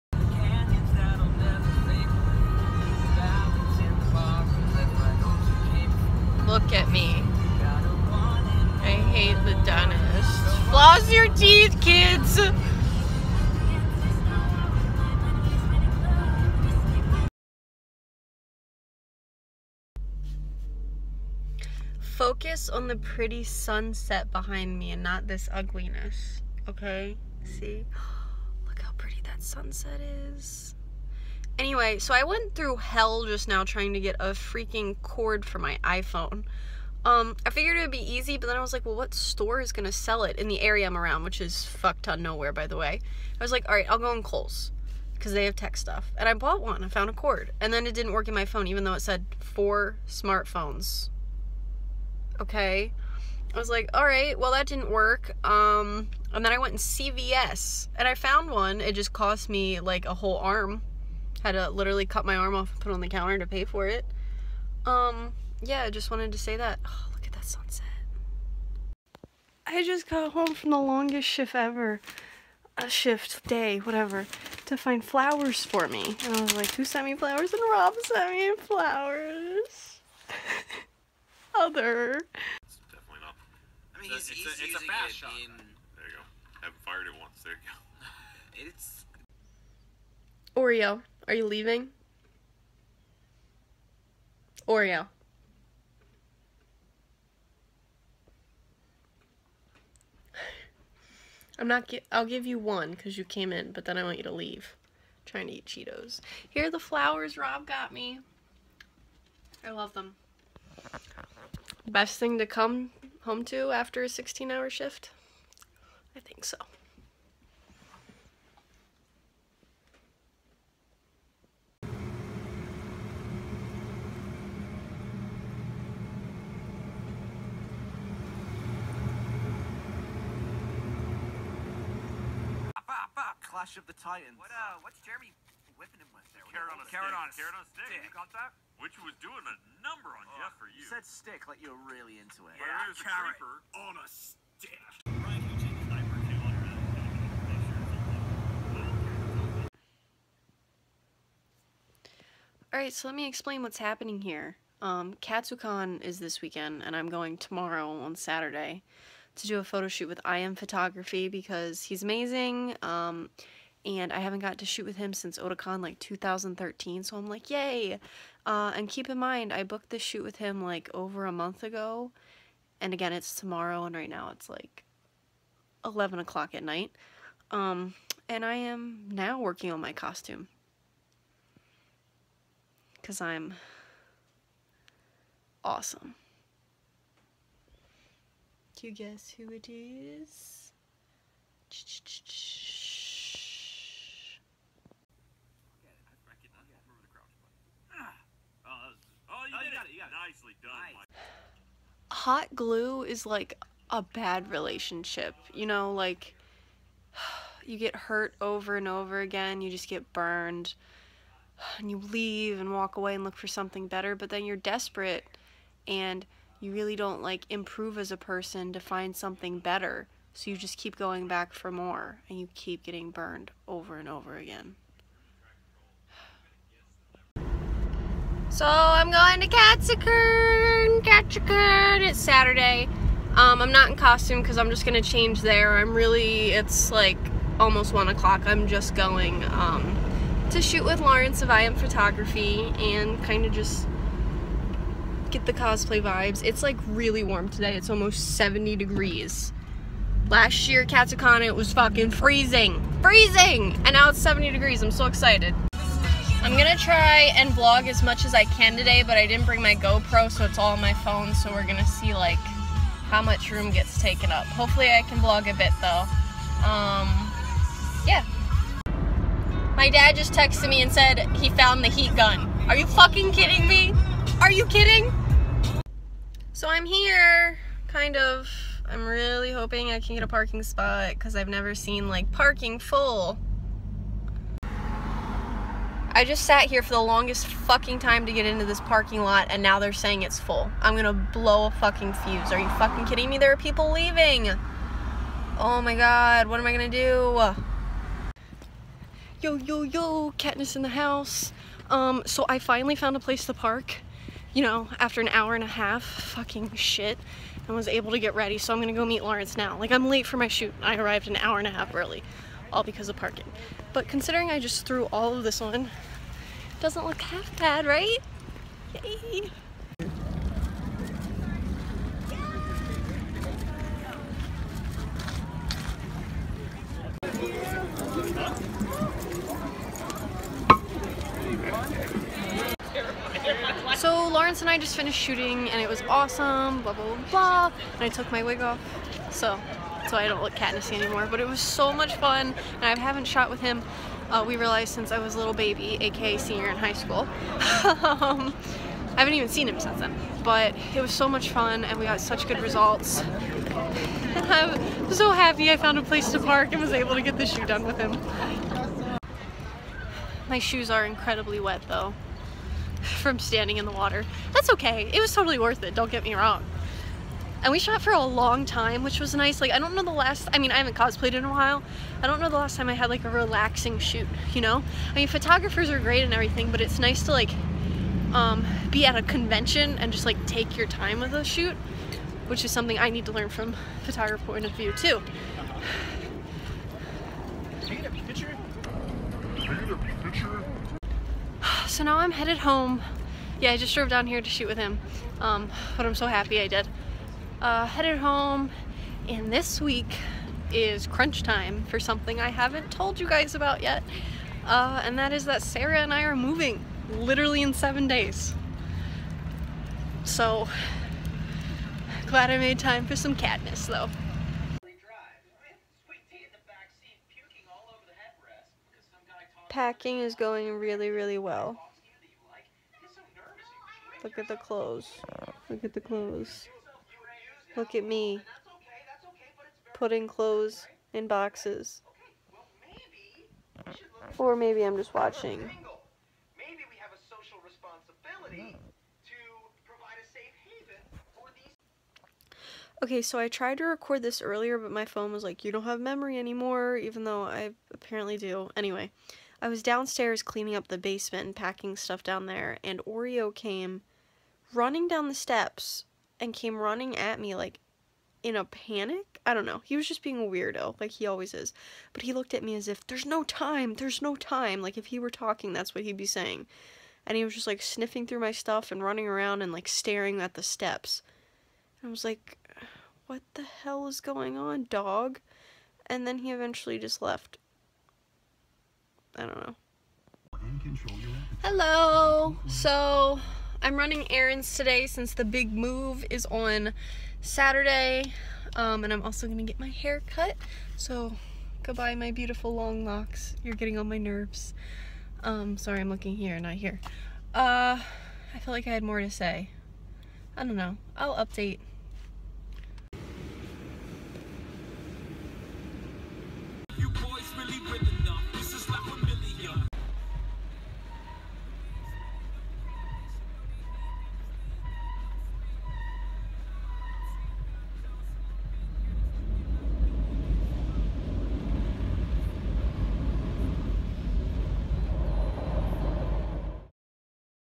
Losers of Cosplay 2: Girl on fire (from hotglue)
Parking con 2017

and the lovely photographer that did a BTS video of us shooting for me!!

MOVING IN 2 DAYS AHHHHHHHHH